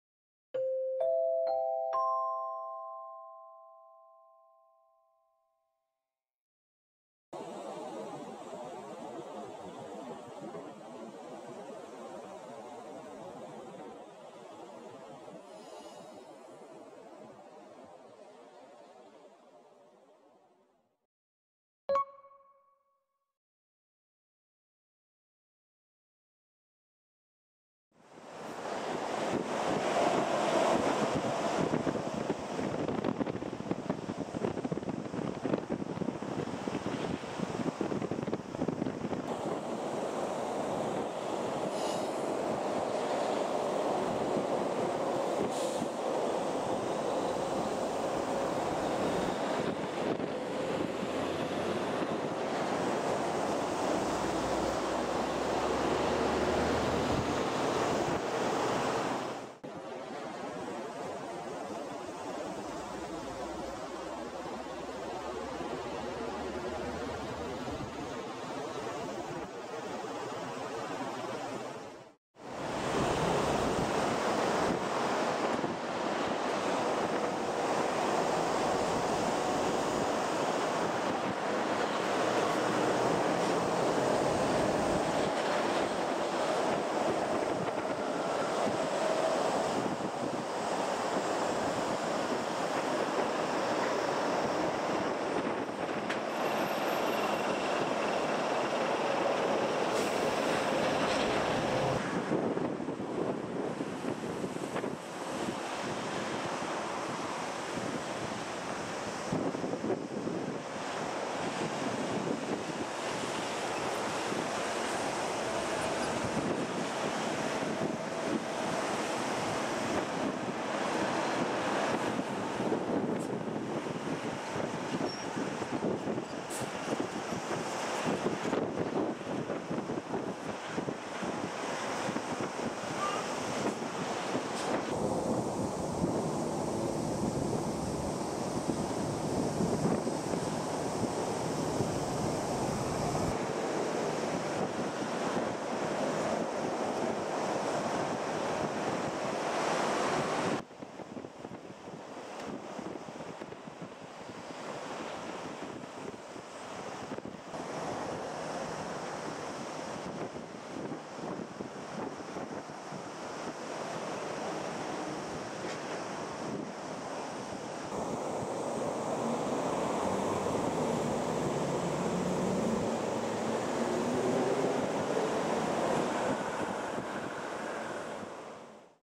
防弾センチュリー＆レクサス警護車軍団 首都高速の大渋滞を行く!! Motorcade of The Japanese Prime Minister
夕方大混雑する首都高速道路に防弾仕様のセンチュリーVIP車と警視庁のレクサス警護車軍団が登場！威圧感満点の鉄壁フォーメーションに周囲の通行車もおっかなビックリ！警護車ファンの皆様向けの動画です！Motorcade of The Japanese Prime Minister